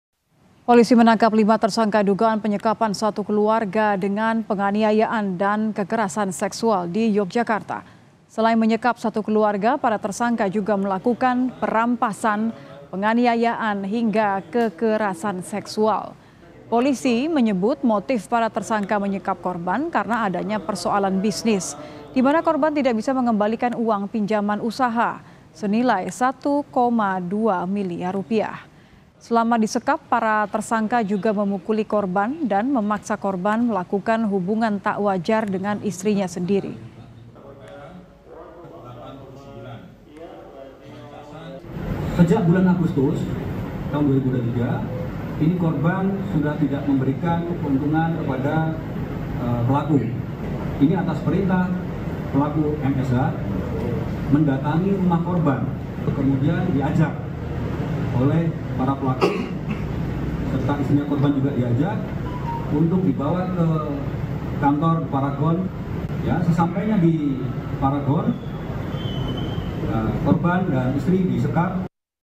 Alasan Persoalan Bisnis, 5 Orang Sekap dan Siksa Pasutri Selama 2 Bulan
SLEMAN, KOMPAS.TV - Polisi tangkap lima tersangka, dugaan penyekapan satu keluarga dengan penganiayaan, dan kekerasan seksual, di Yogyakarta.

Selain menyekap satu keluarga, para tersangka juga melakukan perampasan, penganiayaan, hingga kekerasan seksual.

Baca Juga Sekap dan Aniaya Korban, Pelaku Pencurian Dibekuk Polisi di

Polisi menyebut, motif para tersangka menyekap korban karena adanya persoalan bisnis, di mana korban tidak bisa mengembalikan uang pinjaman usaha, senilai Rp1,2 miliar.

Selama disekap, para tersangka juga memukuli korban, dan memaksa korban melakukan hubungan tak wajar dengan istrinya sendiri.

Kompas TV
Independen | Terpercaya